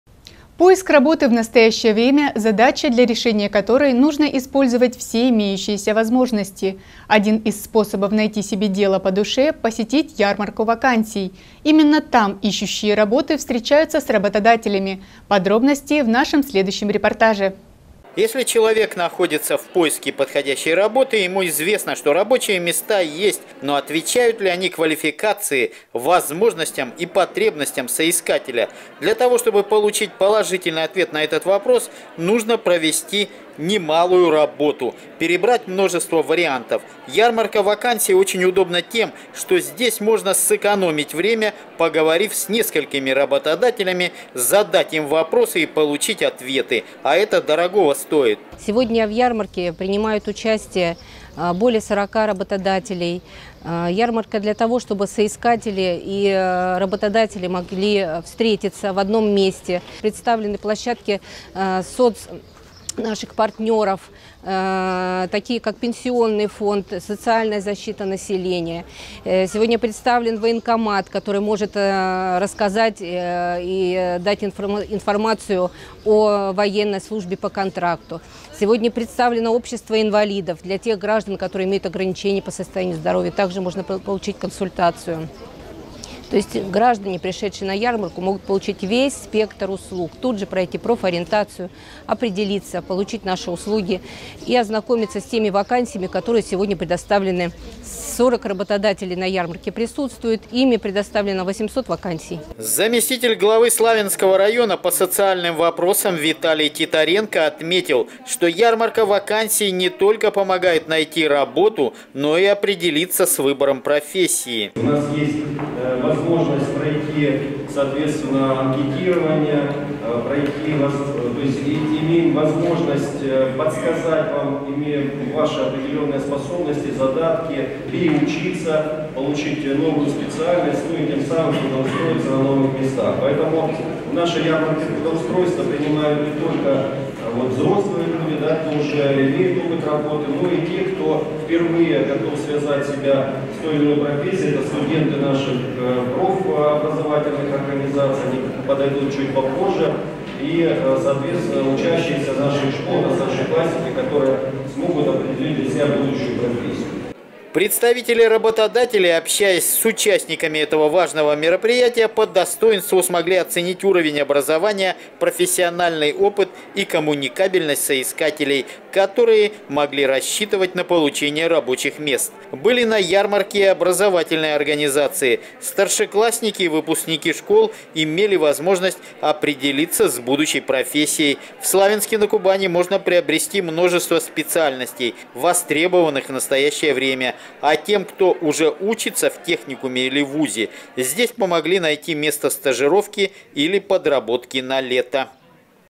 Работа для каждого
Поиск работы в настоящее время – задача, для решения которой нужно использовать все имеющиеся возможности. Один из способов найти себе дело по душе - посетить ярмарку вакансий. Именно там ищущие работу встречаются с работодателями. Подробности – в нашем следующем репортаже